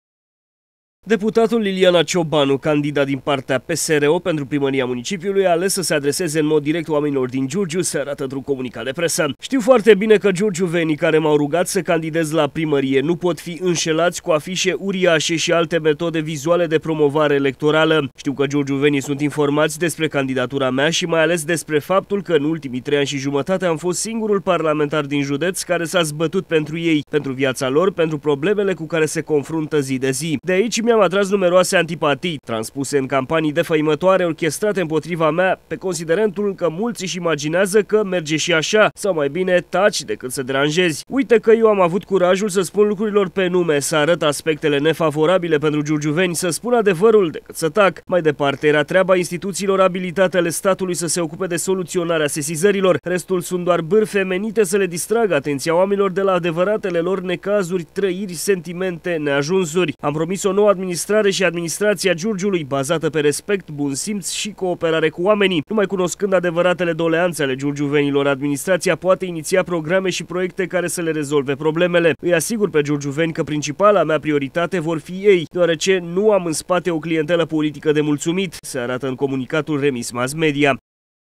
PENTRU O ADMINISTRAŢIE NOUĂ!
DEPUTATUL PSRO LILIANA CIOBANU CONSIDERĂ CĂ„NOUA ADMINISTRAŢIE TREBUIE ORIENTATĂ SPRE ADEVĂRATELE PROBLEME ALE GIURGIUVENILOR!”